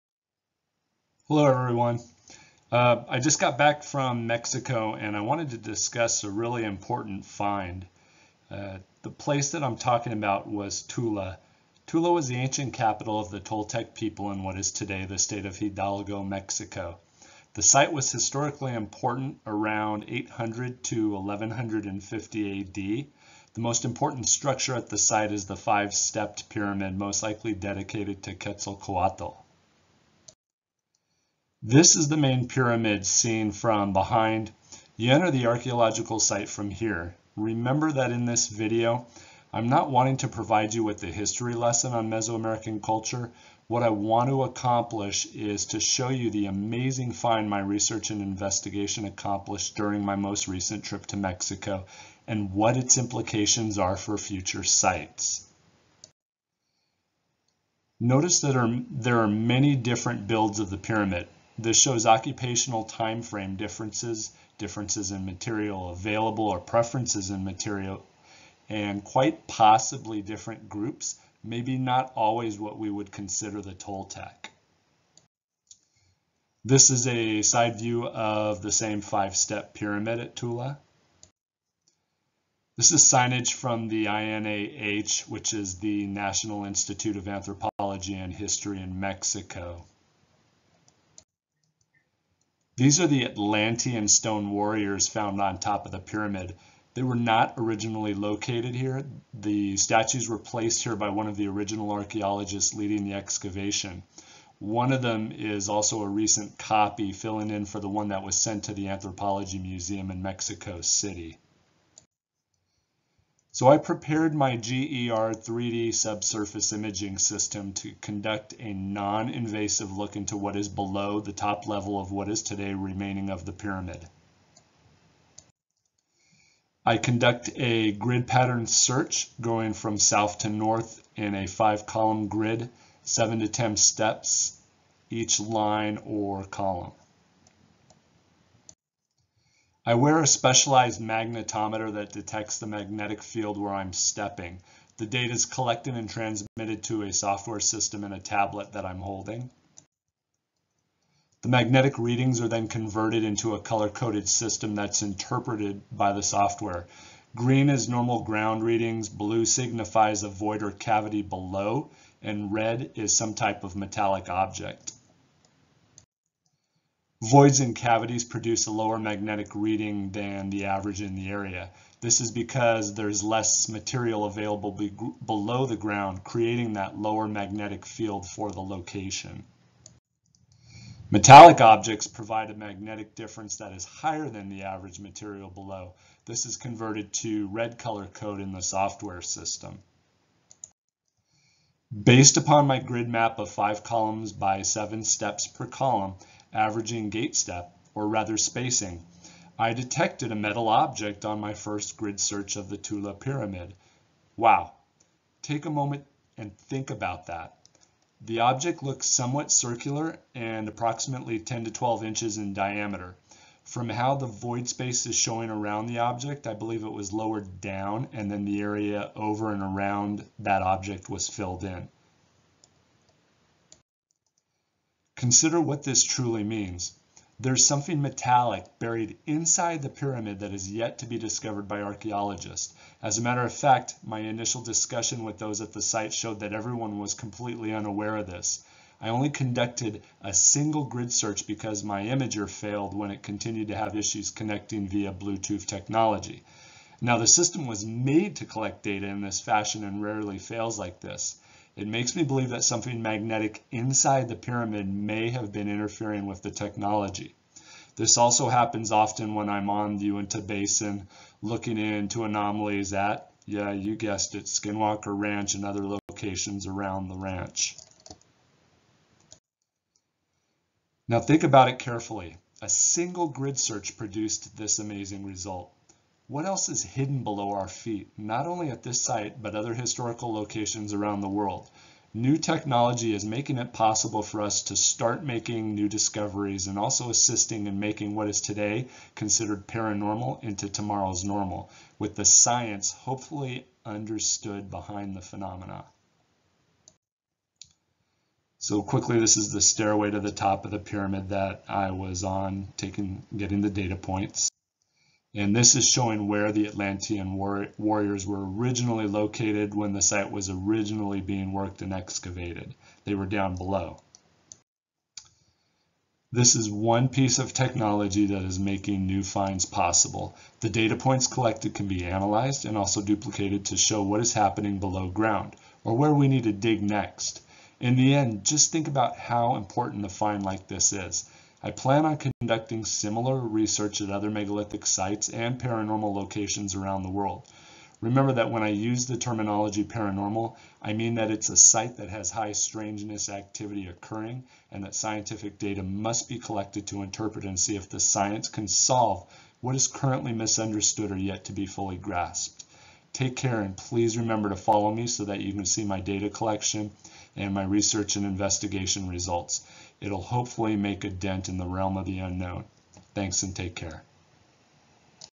Something Hidden Below: 3D Sub-Surface Imaging At The Ancient Site Of Tula
Using a 3D Sub-Surface Imaging Magnetometer, a new find was found below the upper portion of the Pyramid at Tula. The object was detected on a solo grid search. The implications of such a find are tremendous, not just at this site but at other ancient sites and even paranormal locations.

Using non-invasive technology, what else can be detected that has been hidden away, beneath our feet? What will these finds mean for our history? What will this technology do to our understanding of the unknown and areas of high strangeness?